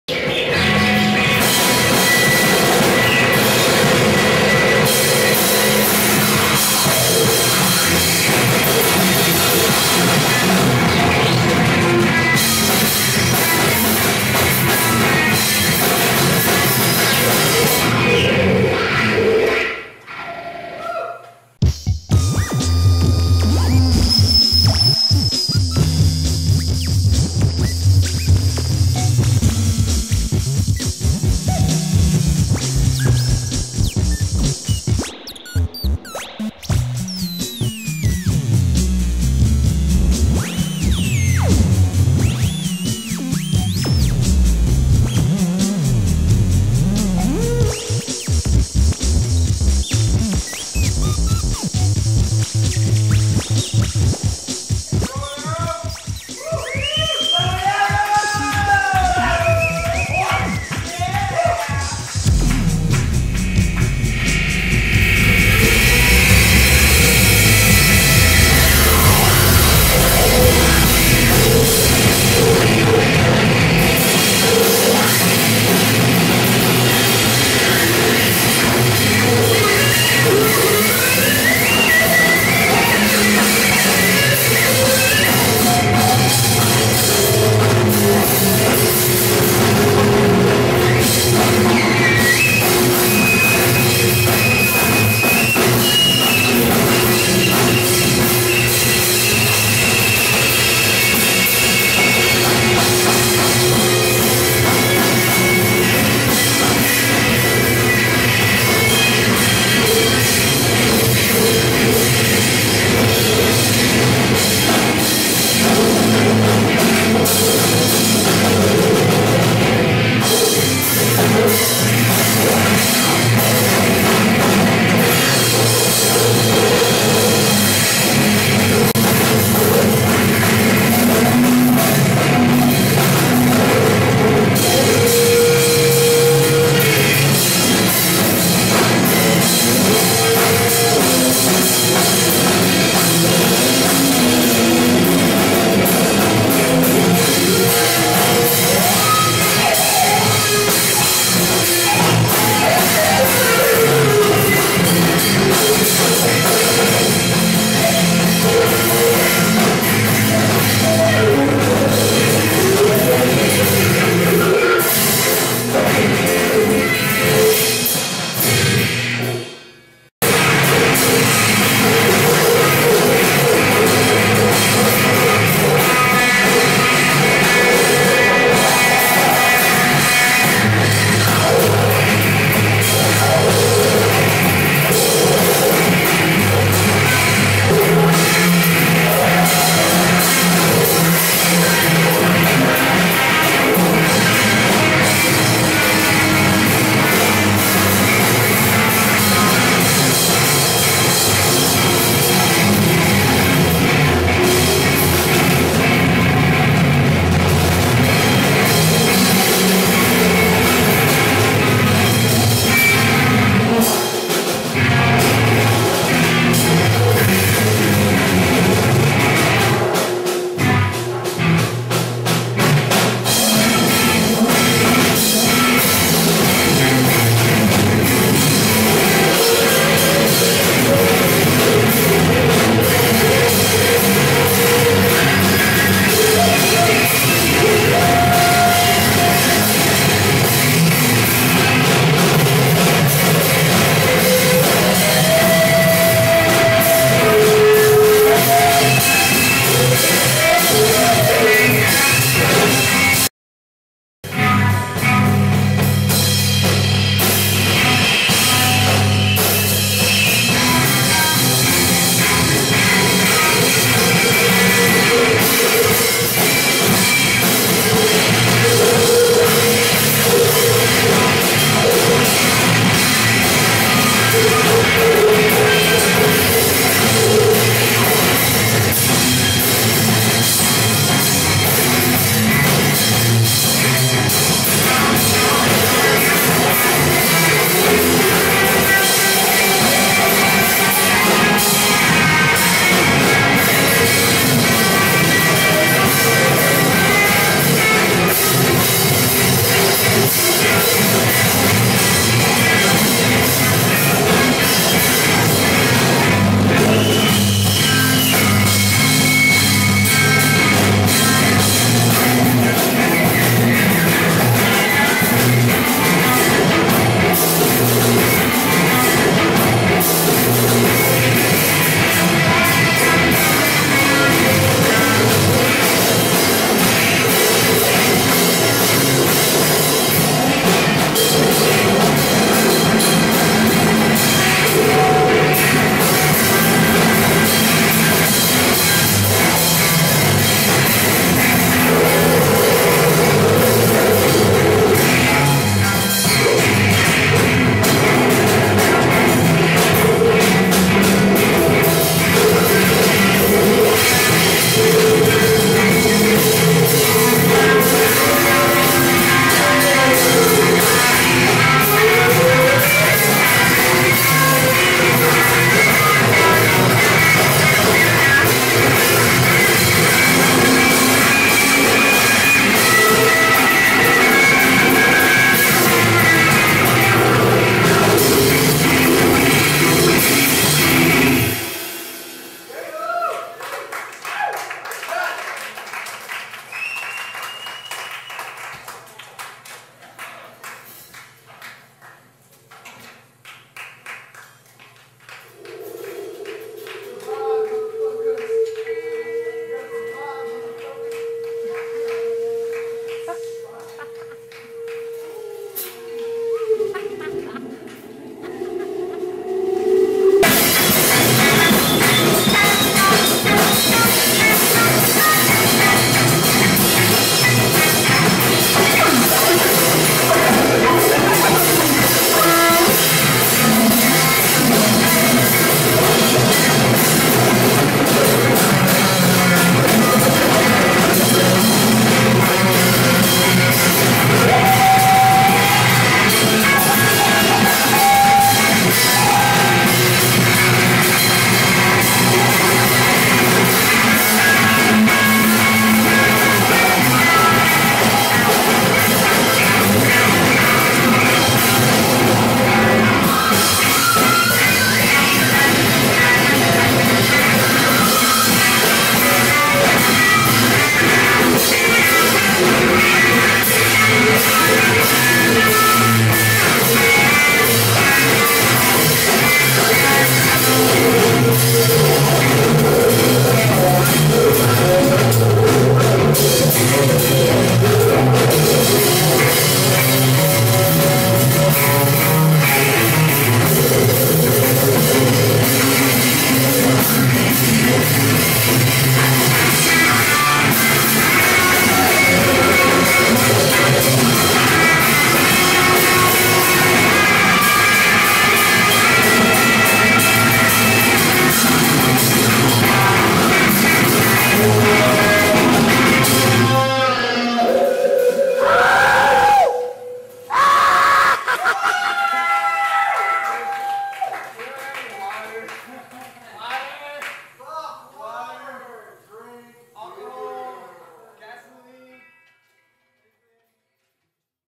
Bow & Arrow @ Hatch Gallery
avant-garde chaotix from Los Angeles' Bow & Arrow, live at the Hatch Gallery in Pasadena, 2006.